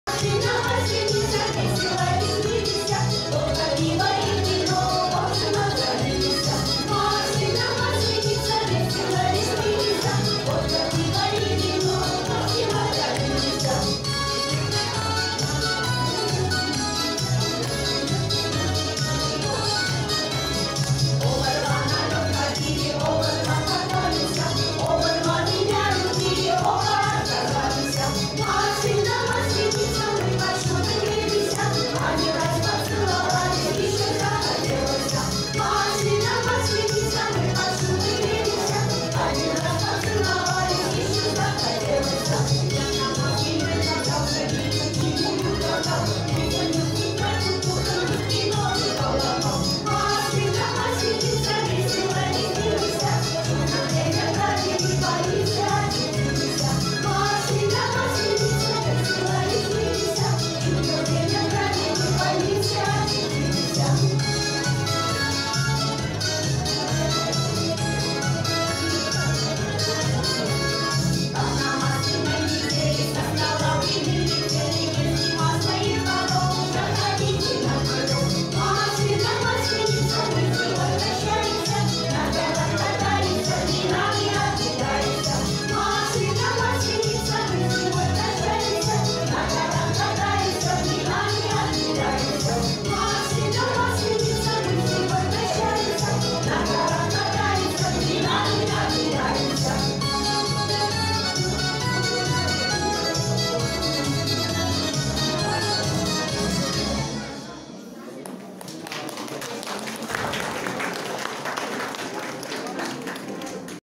Хор "Рябинушки" г. Степанаван
В Русском Доме г.Гюмри отметили Масленицу .